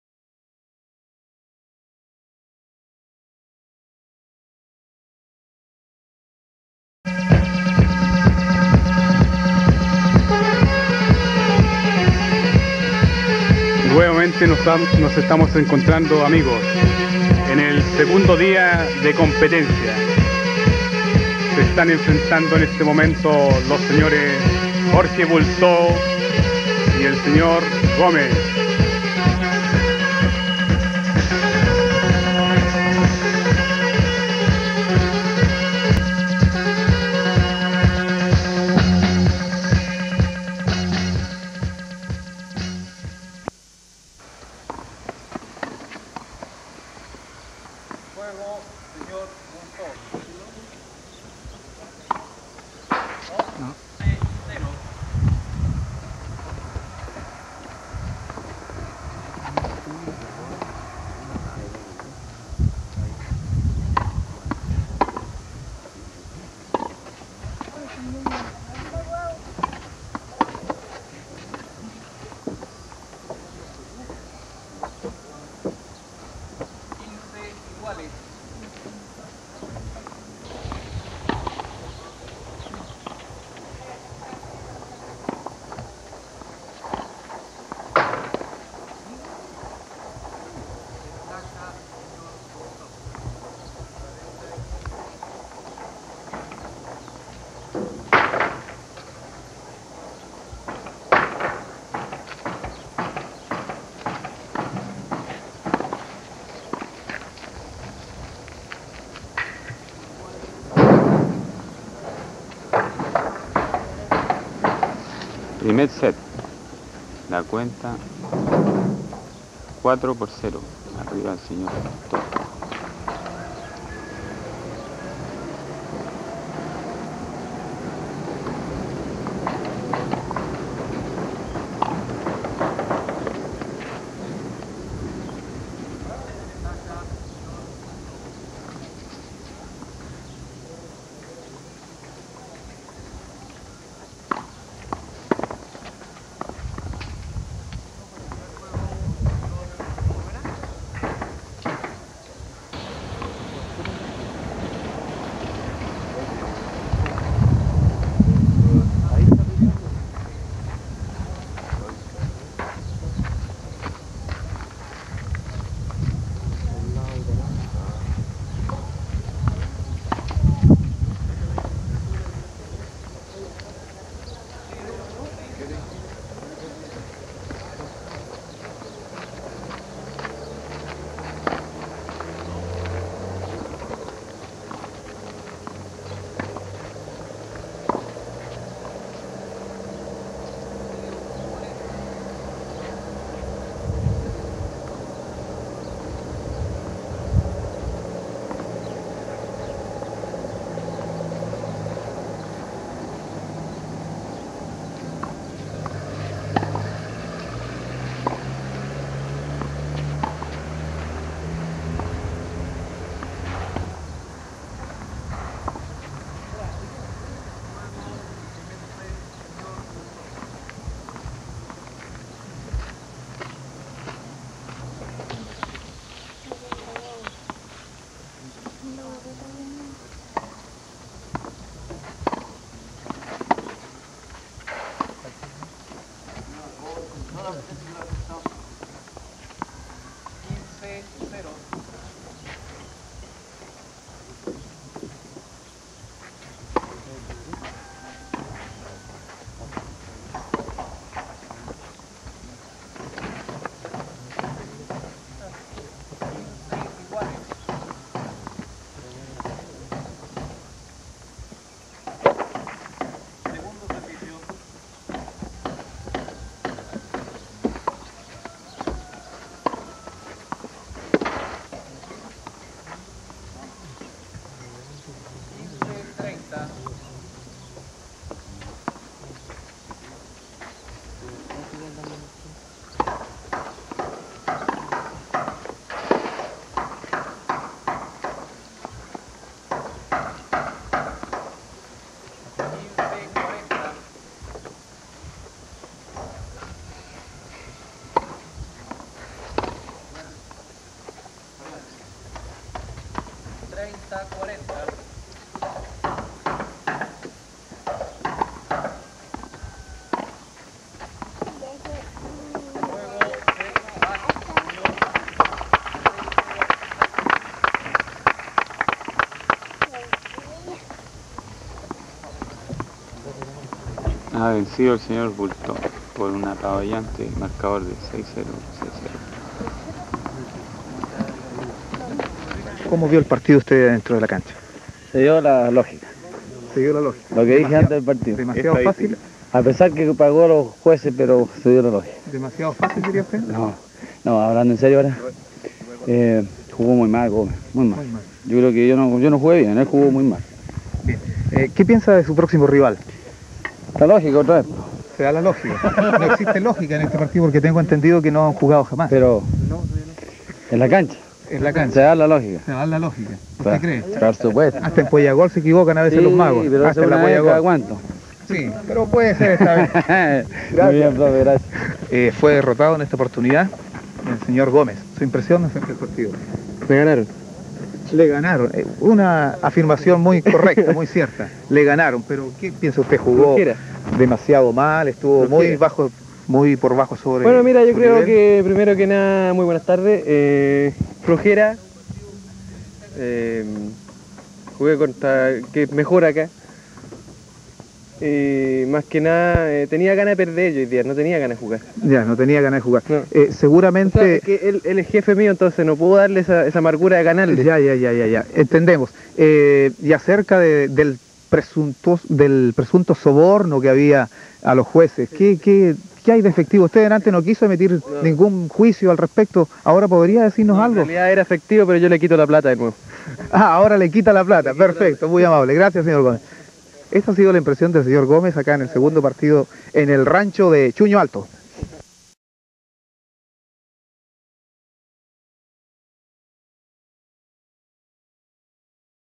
1987 12 25 Campeonato Tenis 06 Jorge Bulto vs Andrés Gomez CASR
Campeonato Navidad 1987 de Tenis Regimiento Miraflores Traiguén